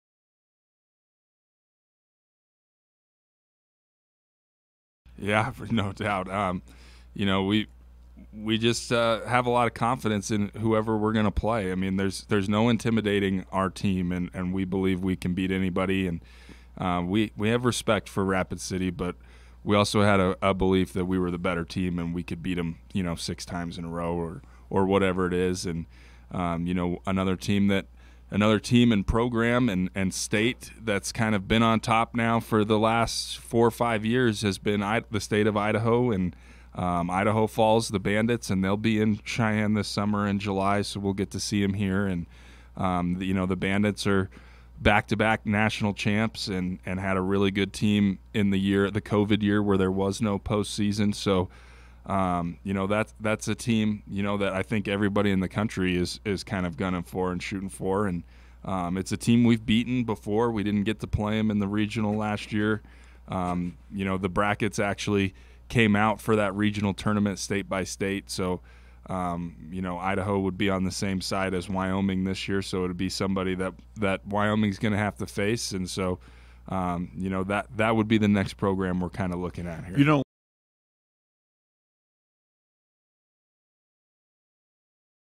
The ProShow: Cheyenne Post 6 Manager Ty Lain
ICYMI: Cheyenne Post 6 Manager Ty Lain talks about elevating the program and who he sees as the next program out there he wants to surpass on the level of success.